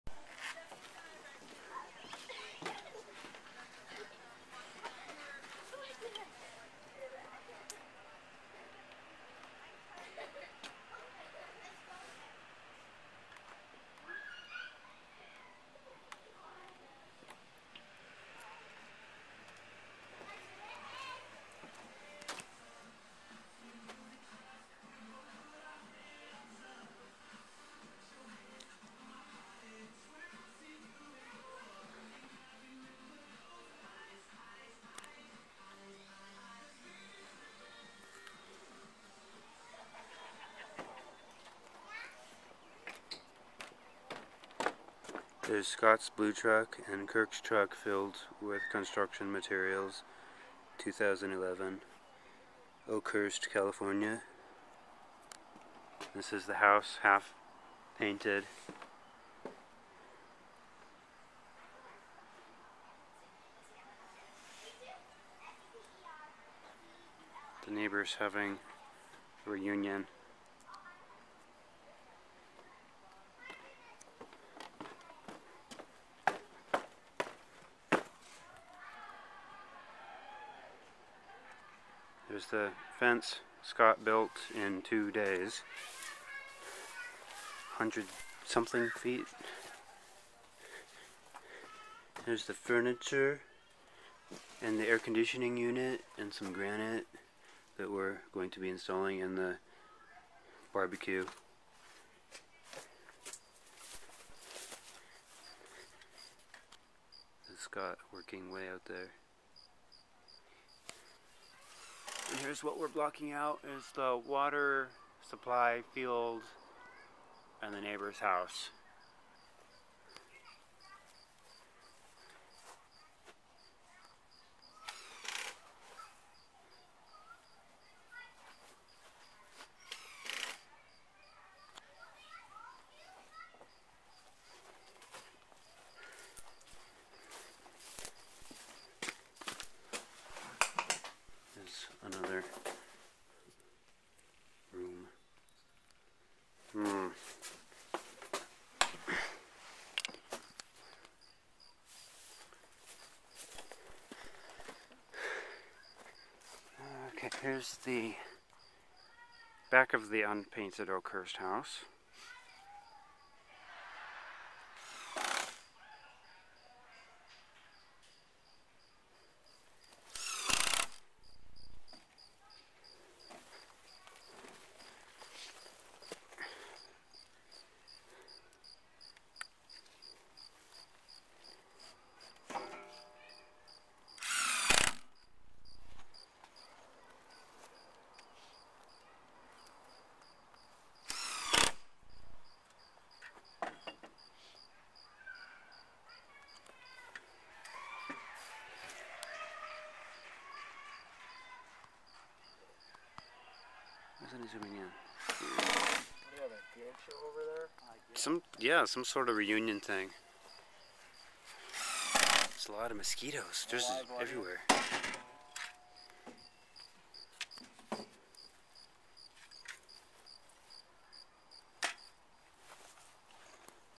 Oakhurst CA Construction Progress
This is the house I helped renovate. The video shows the home half way complete. Video taken the summer of 2011. Oakhurst California.

The neighbors were having some kind of reunion.